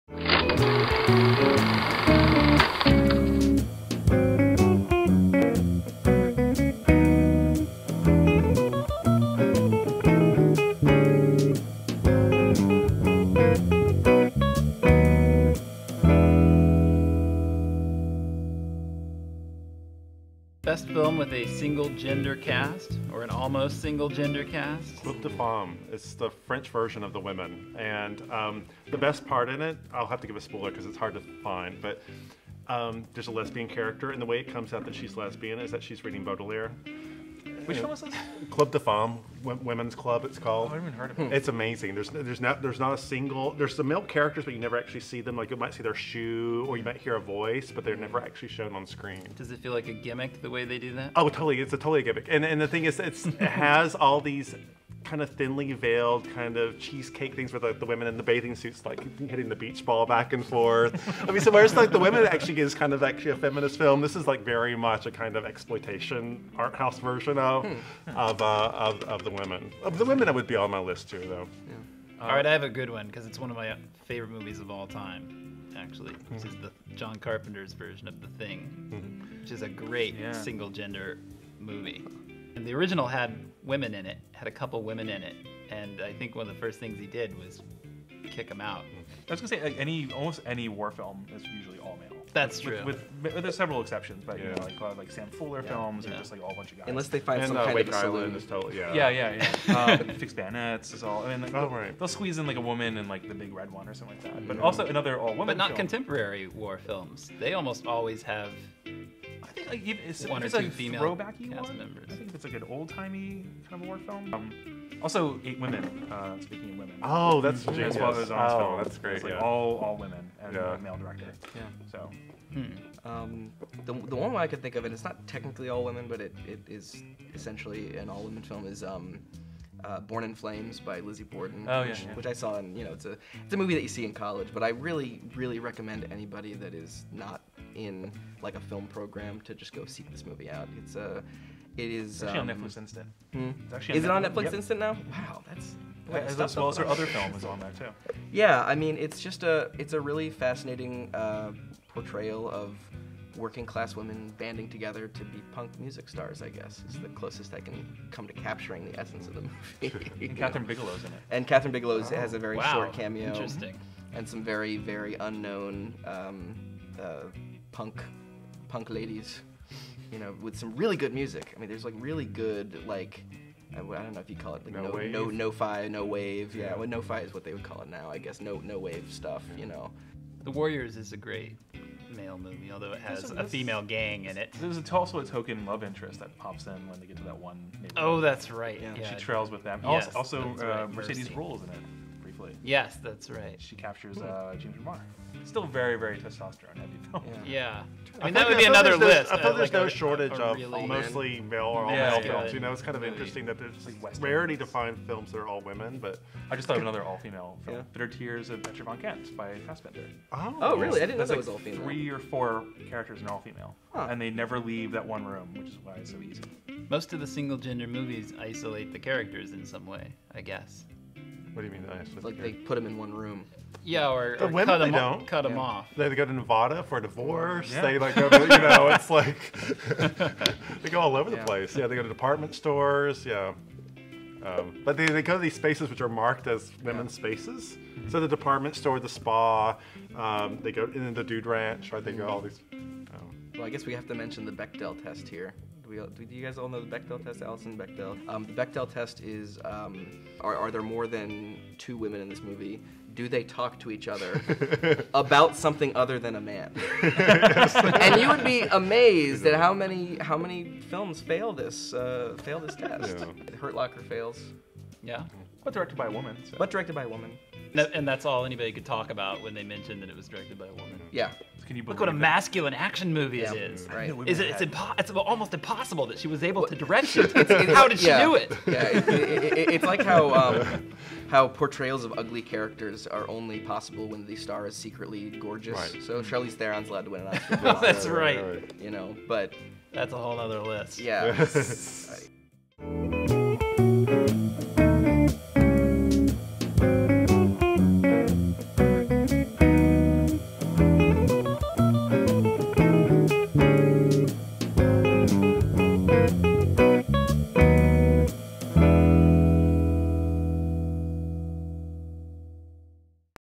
Best Film with a Single-Gender Cast
On this week's episode of Film Versus Film, the panel discusses films with single-gender or almost single-gender casts.  Among the films discussed are the little-seen French film Club de Femmes ("Women's Club"), the classic The Women (George Cukor), 8 Women (Ozon), Born in Flames (Lizzie Borden), a punk rock drama featuring the Oscar-winning director Kathryn Bigelow in a cameo appearance, The Warriors (Walter Hill), and The Bitter Tears of Petra Von Kant. Also discussed is the Bechdel Test for films, which has the following rules
1. It has to have at least two [named] women in it
2. Who talk to each other
3. About something besides a man